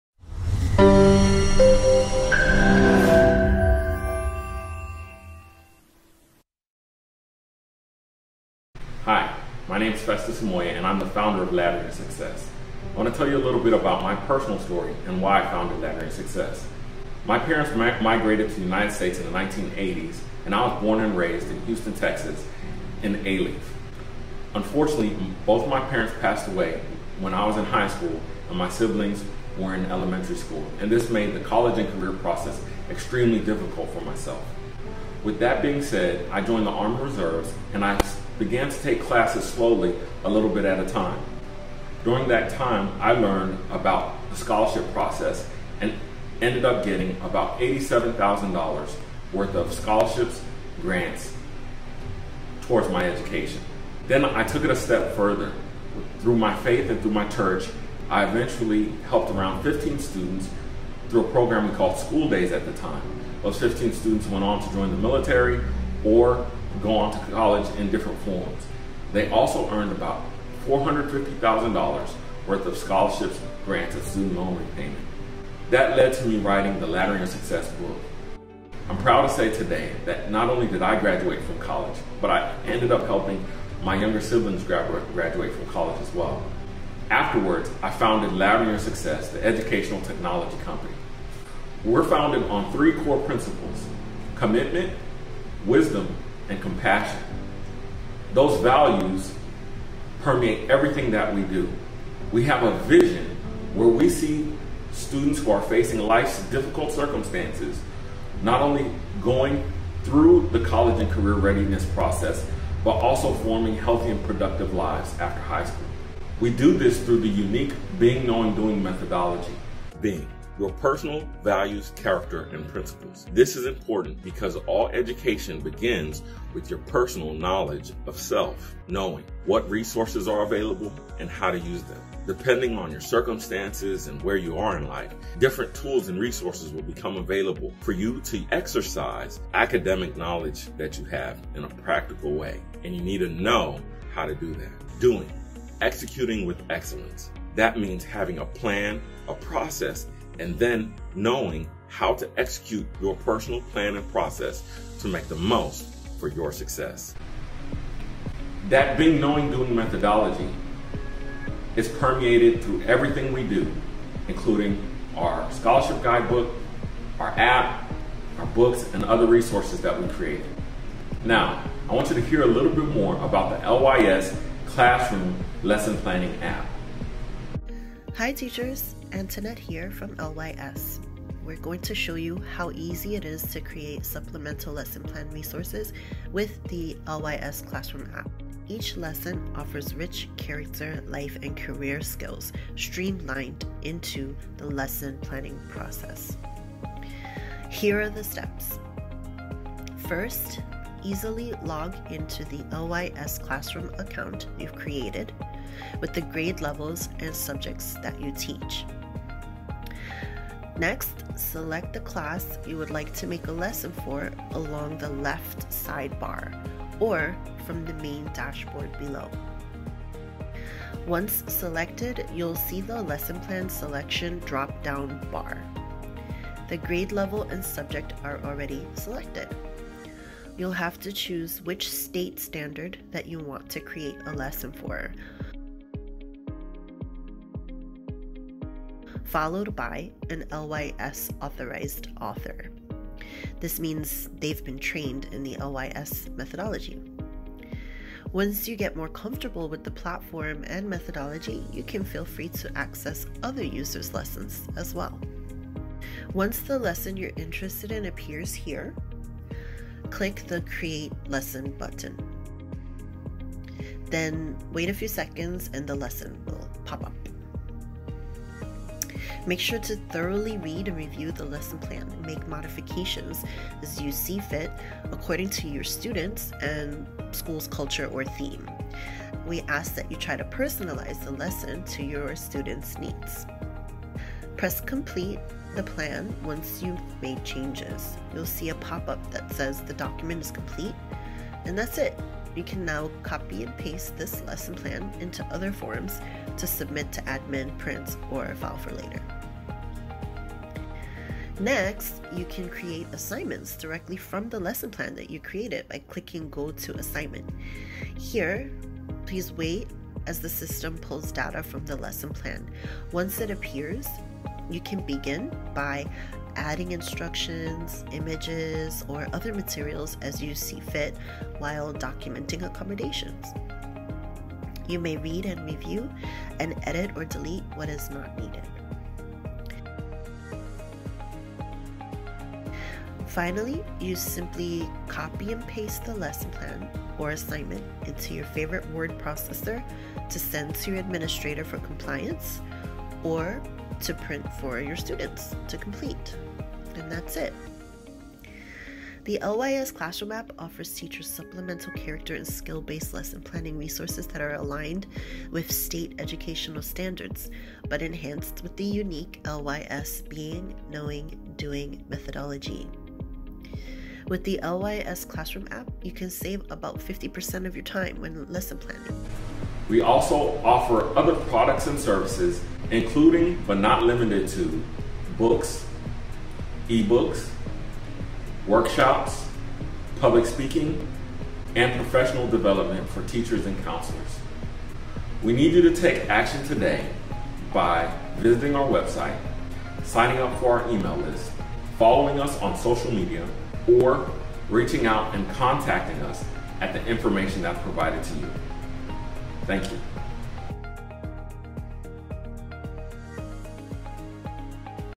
Festus' Journey to LYS
Laddering Your Success was born through its ceo and founder's life experiences. Festus Amoye describes his personal journey to success, how LYS came to be, and shares what LYS is about while giving product demonstrations!